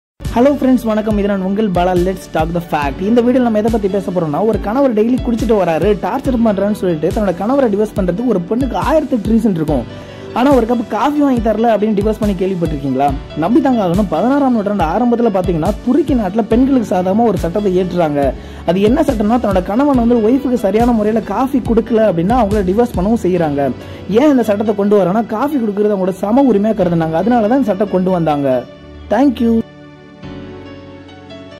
Divorce for a cup of Coffee
16 ஆம் நூற்றாண்டில் துருக்கி நாட்டில் ஒரு கணவர் தனது மனைவிக்கு காபி வழங்காவிட்டால் அது விவாகரத்து செய்வதற்கான சட்டபூர்வமான காரணமாகும்